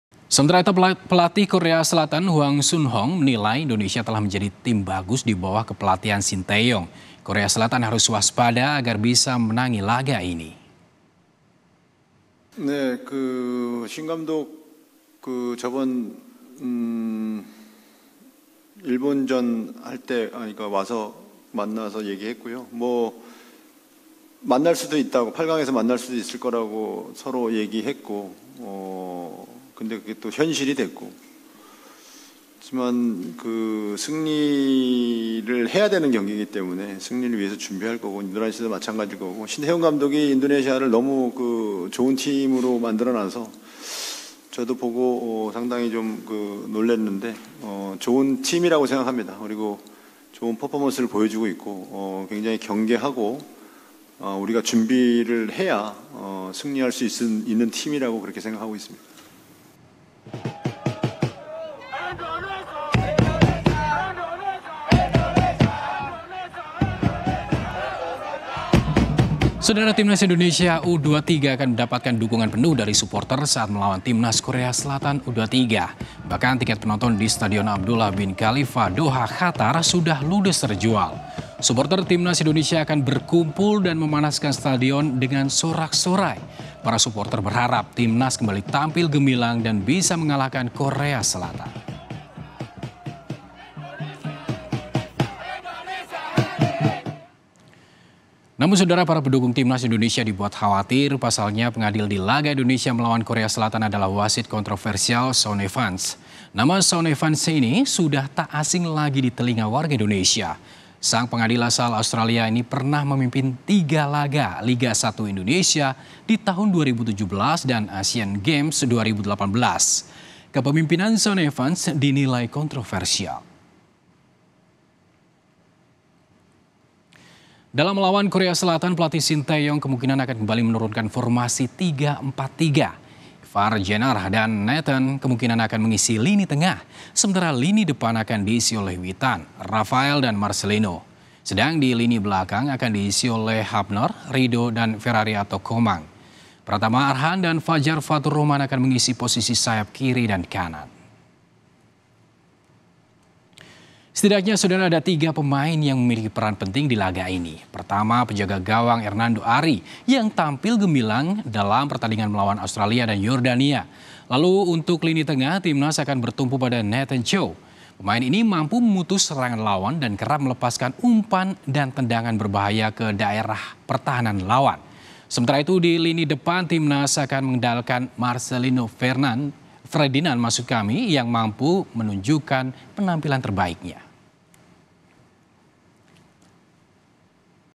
Pelatih Korsel: Dilatih Shin Tae Yong, Indonesia Tim yang Bagus!
KOMPAS.TV-Pelatih Korsel: Dilatih Shin Tae Yong, Indonesia Tim yang Bagus!